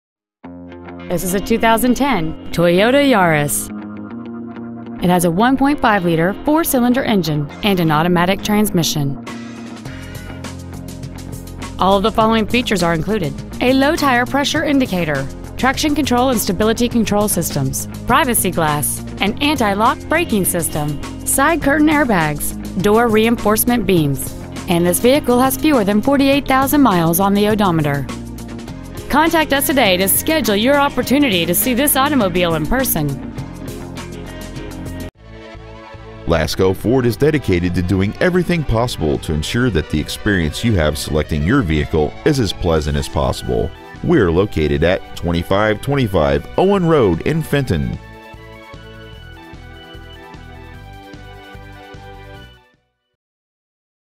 2010 Toyota Yaris Fenton MI
Year : 2010
 Make : Toyota
 Model : Yaris
 Engine : 1.5L I4 16V MPFI DOHC
 Trans . : AUTOMATIC
 Exterior : SILVER
 Miles : 47,234
 Interior : DARK CHARCOAL